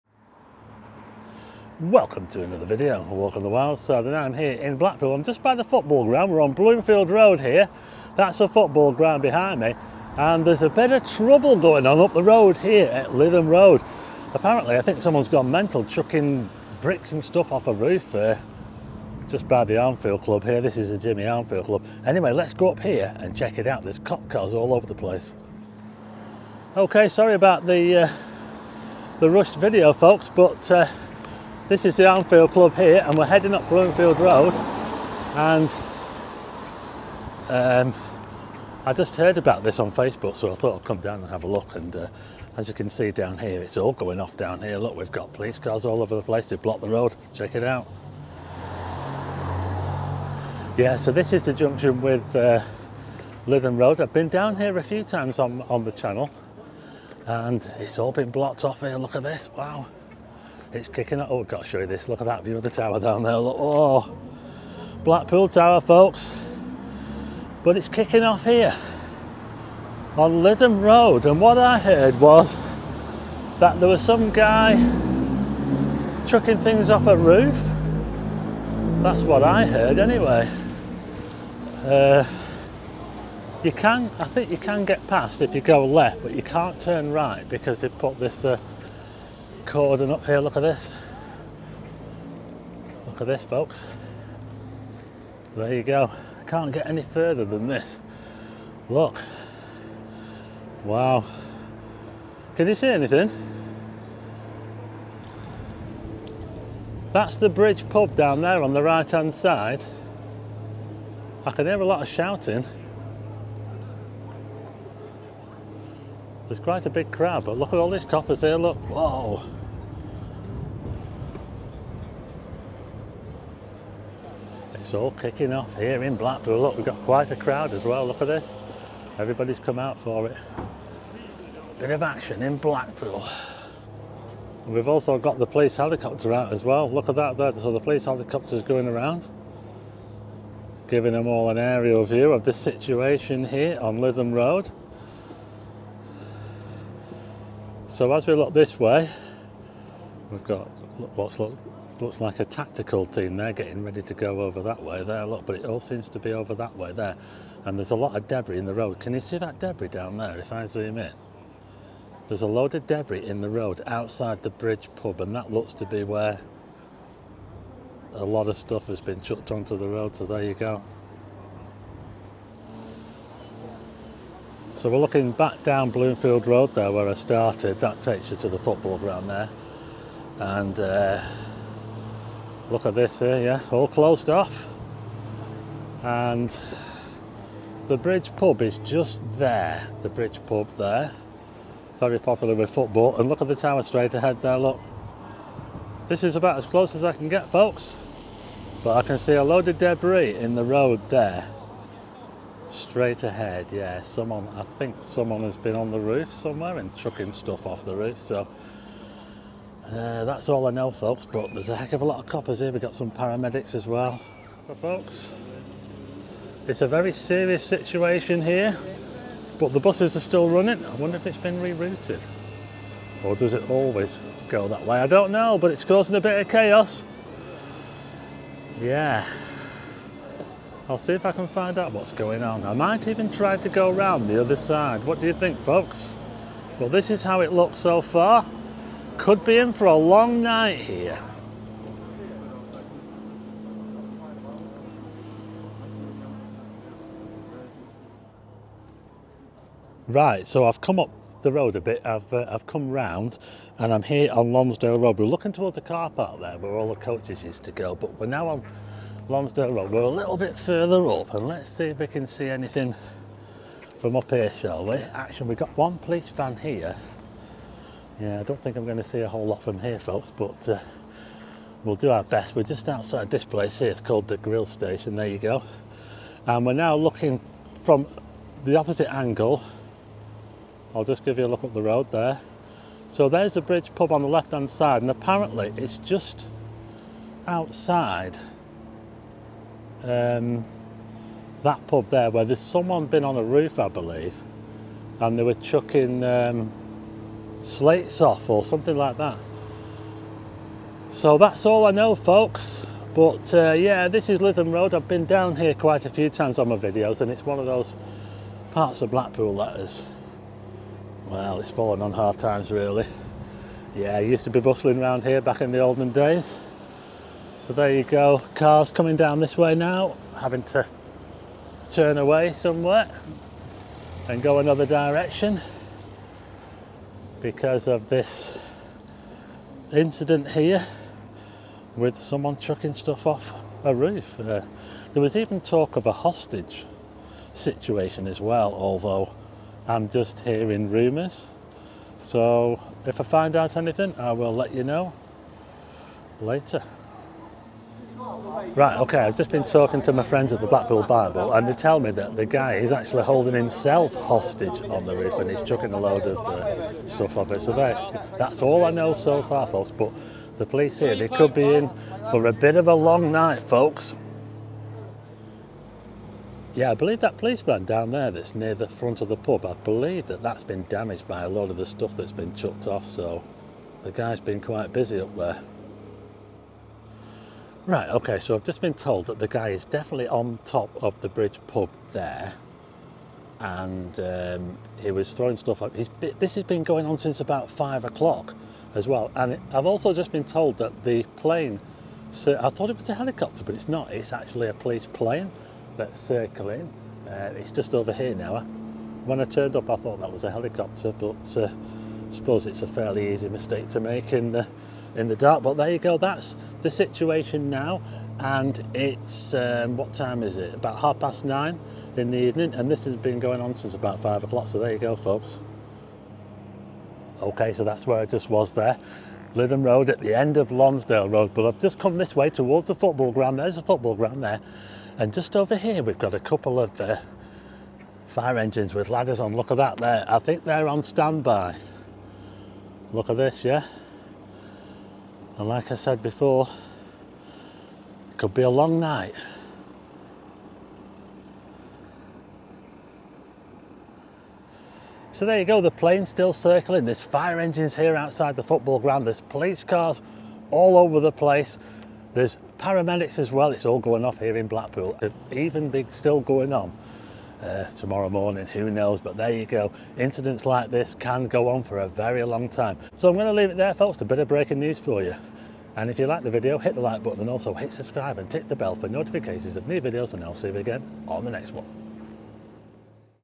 Police👮‍♂️Incident Blackpool: Man on Roof
Statement from police:
We are currently dealing with an incident in Lytham Road, Blackpool, where a man has climbed on top of a building and is throwing objects off the roof.
Our immediate priority is his safety and that of the public. We have closed surrounding roads to allow us to deal with this incident.
Please avoid the area. We will update you when we can.